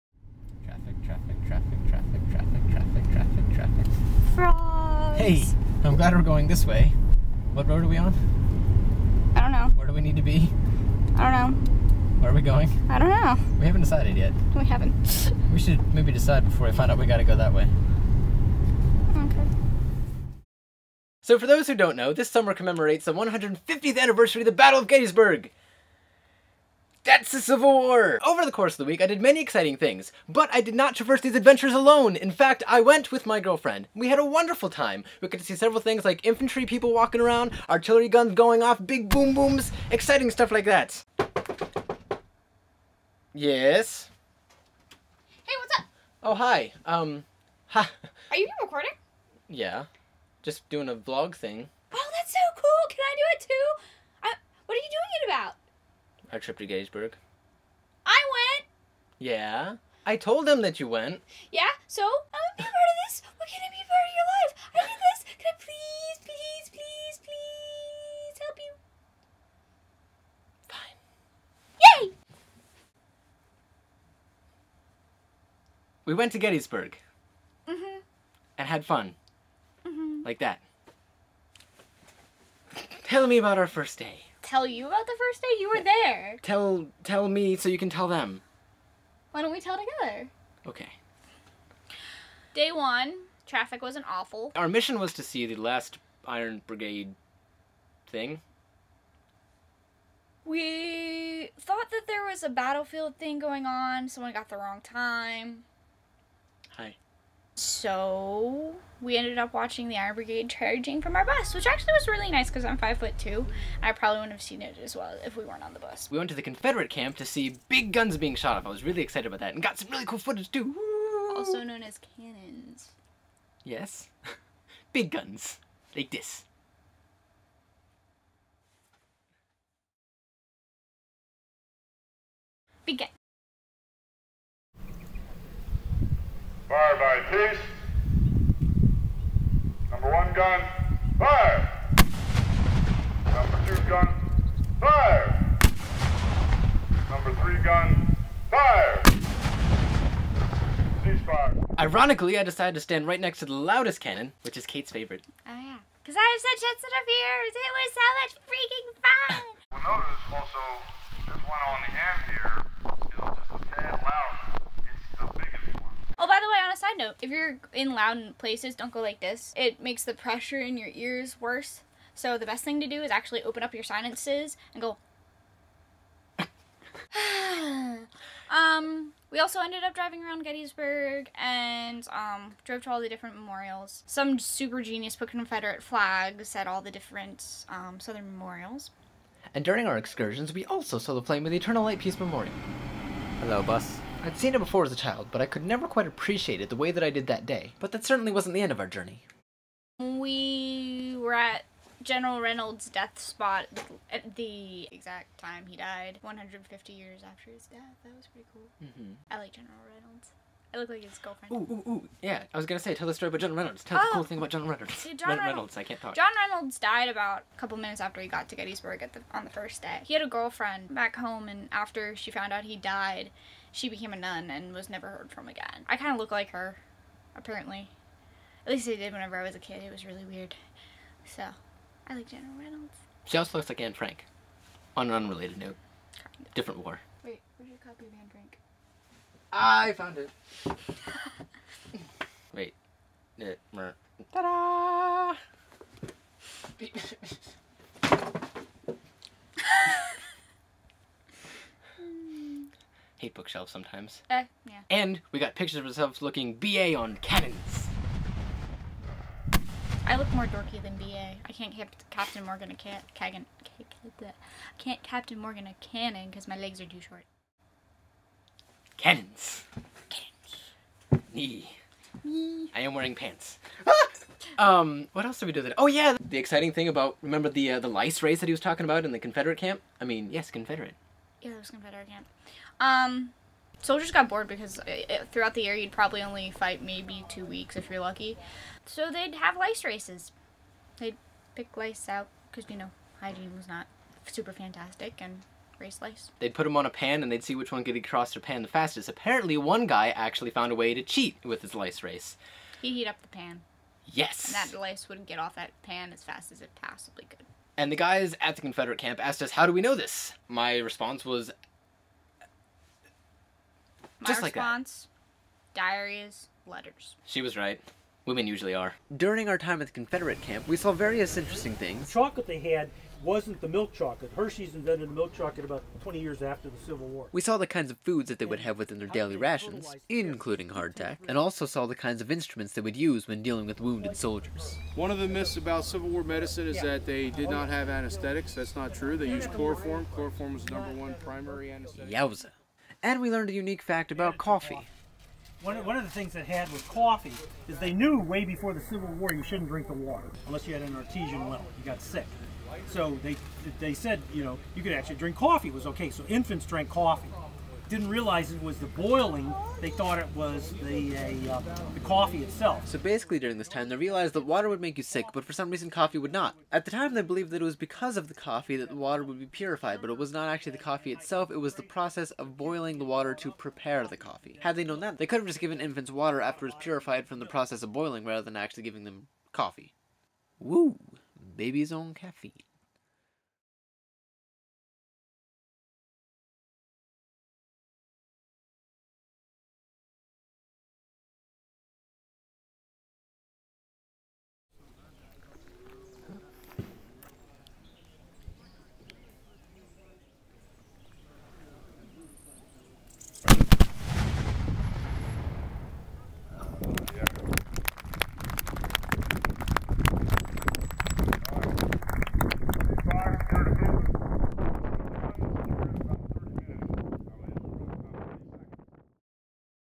Celebrating Gettysburg | 150 Ways to Fail as a Tourist - Part One
For the 150th anniversary of the Battle of Gettysburg, we took a trip to see what we could see.  This video is of our adventures and the things we saw on July 1, 2013 [the anniversary of the first day of battle].  The main events of this day include: A Confederate Canon Demonstration, and a view in the lives of the Confederates in their campsite.